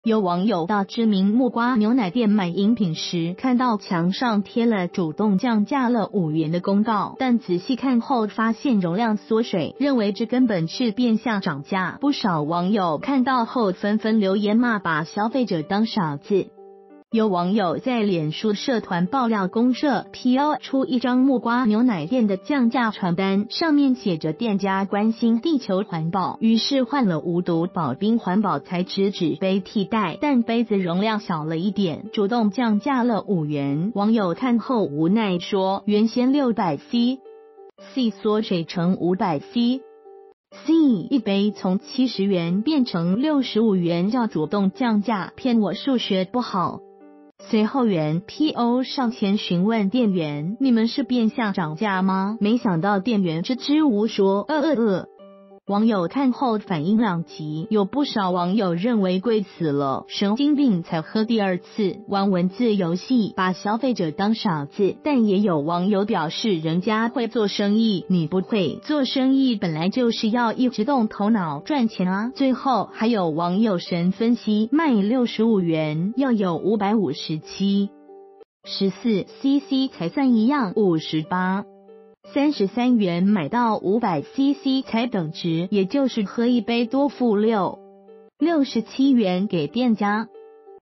根本多付6.67塊！手搖店主動降5元 遭揭穿是變相漲價
有網友到知名木瓜牛奶店買飲品時，看到牆上貼了「主動降價了5元」的公告，但仔細看後發現容量縮水，認為這根本是「變相漲價」。不少網友看到後紛紛留言罵「把消費者當傻子」！

發表評論評論如下。